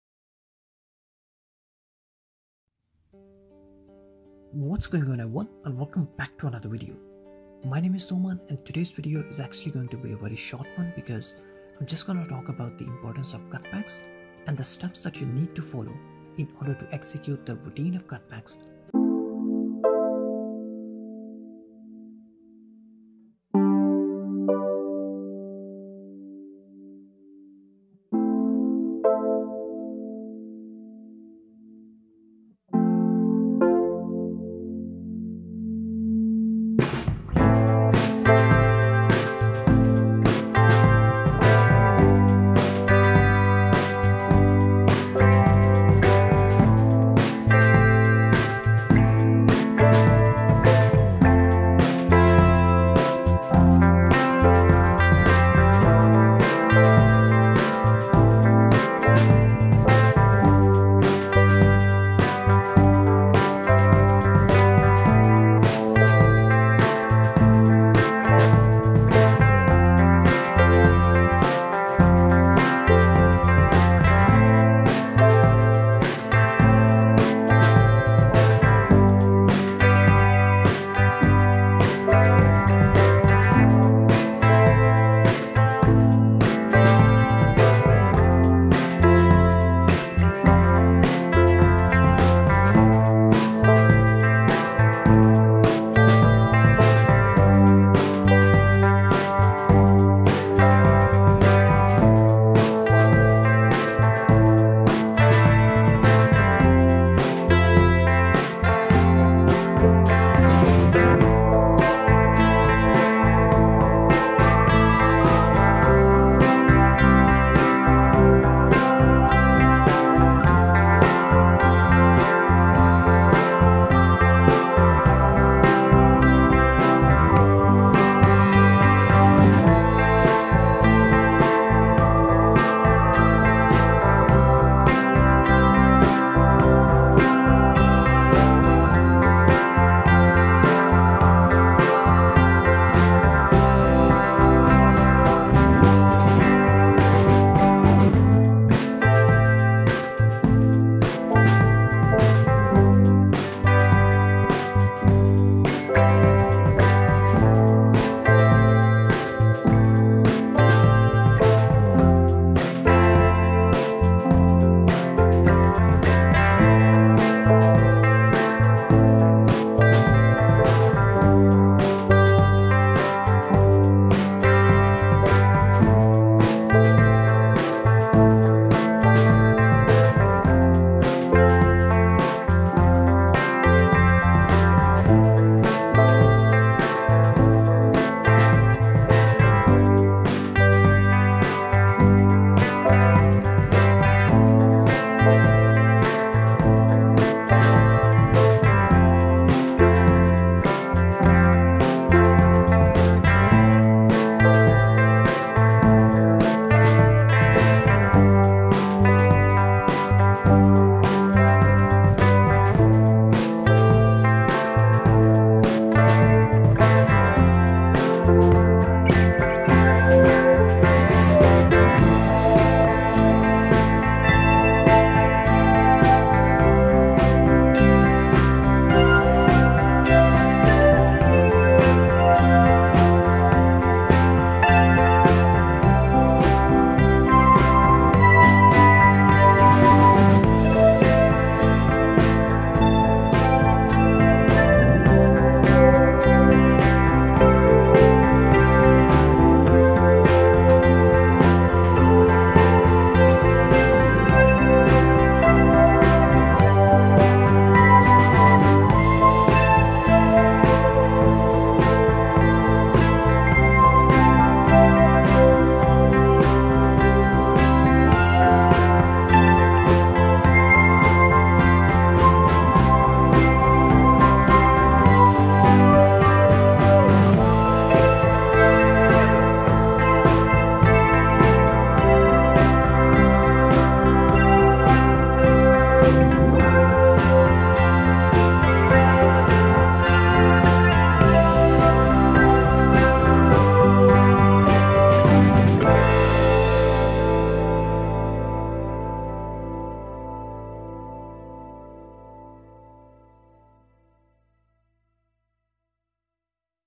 The Art of Cut-backs ft. Arsenal and Man City - Football Basics
In today's video, I've explained the steps that you need to follow in order to execute the routine of cut-backs perfectly. I've used examples from Arsenal and Man City's game-play as both these clubs use cut-backs extensively